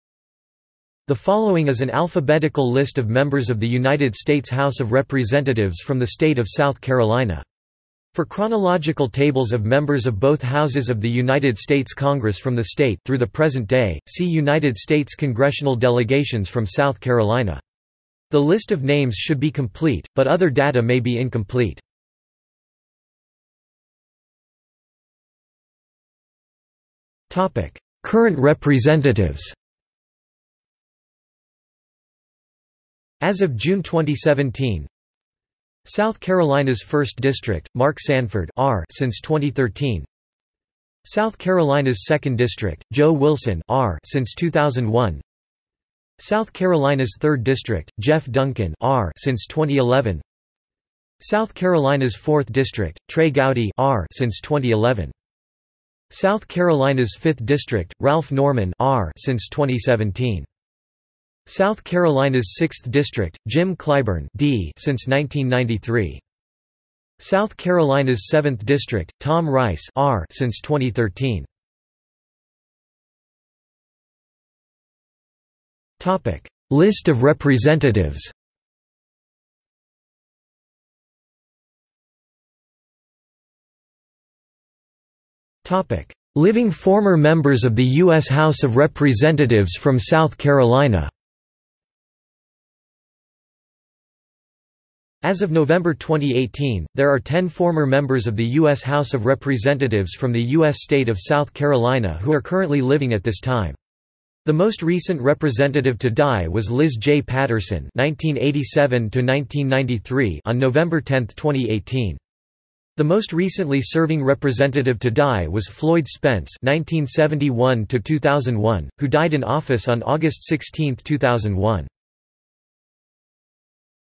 List of United States Representatives from South Carolina | Wikipedia audio article
This is an audio version of the Wikipedia Article:
List of United States Representatives from South Carolina

00:00:23 1 Current representatives
00:01:12 2 List of representatives
00:01:22 3 Living former Members of the U.S. House of Representatives from South Carolina

- Socrates

SUMMARY
The following is an alphabetical list of members of the United States House of Representatives from the state of South Carolina.  For chronological tables of members of both houses of the United States Congress from the state (through the present day), see United States Congressional Delegations from South Carolina. The list of names should be complete, but other data may be incomplete.